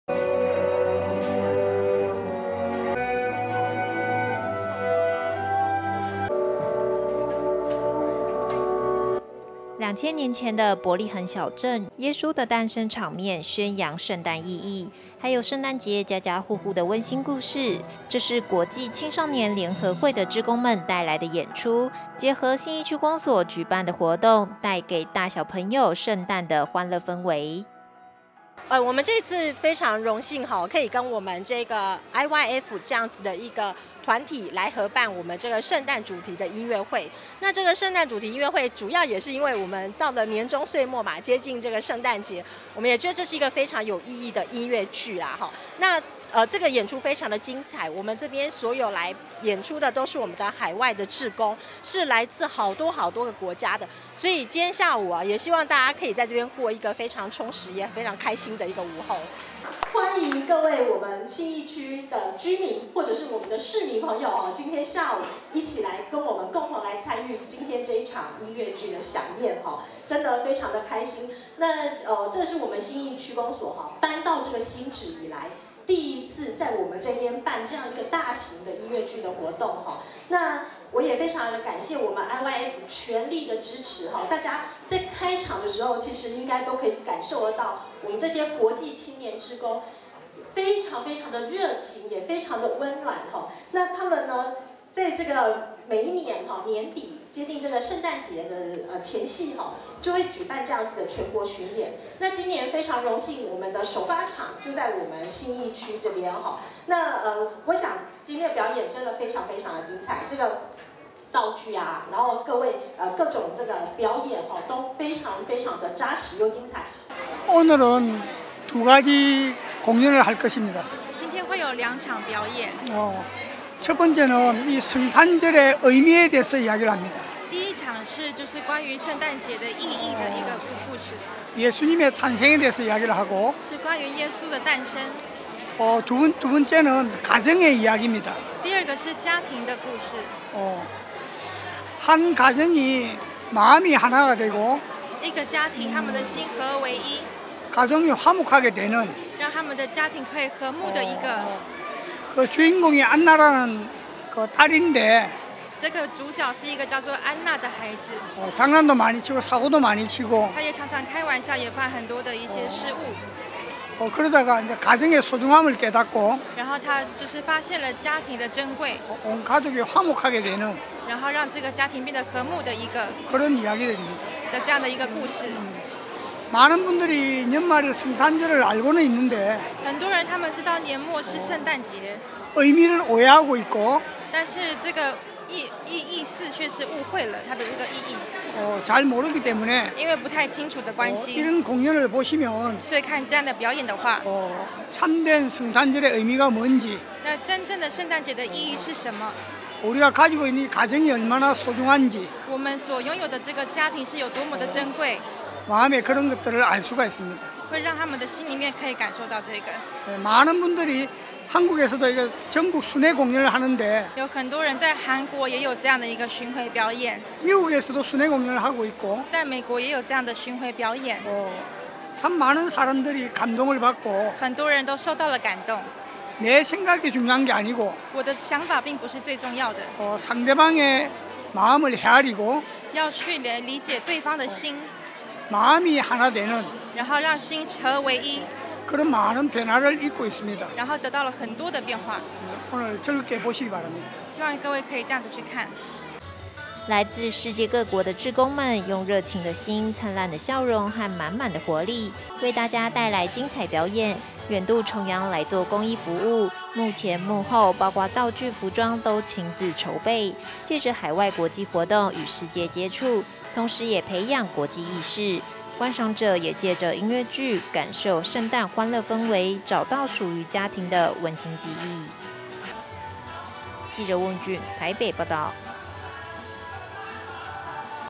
聖誕節|台北|聖誕主題音樂劇 世界各地青年志工聯合演出
聖誕主題音樂劇 世界各地青年志工聯合演出

聖誕節佳節逐漸來臨，在充滿感恩與溫馨的時節，信義區公所與臺灣國際青少年聯合會舉辦2023聖誕主題音樂劇，來自世界各國的志工，用滿滿的能量與熱情為大家帶來精彩演出。

兩千年前的伯利恆小鎮，耶穌的誕生故事，宣揚聖誕意義，還有聖誕節家家戶戶的溫馨故事，這是國際青少年聯合會的志工們的演出，結合信義區公所，帶給大小朋友聖誕的歡樂氛圍。

來自世界各國的志工們，用熱情的心、燦爛的笑容和滿滿的活力，為大家帶來精彩表演，遠渡重洋來做公益服務，幕前幕後，包括道具服裝都親自籌備，藉著海外國際活動，與世界接觸，同時也培養國際意識。觀賞者也藉著音樂劇感受聖誕歡樂氣氛，找到屬於家庭的溫馨記憶。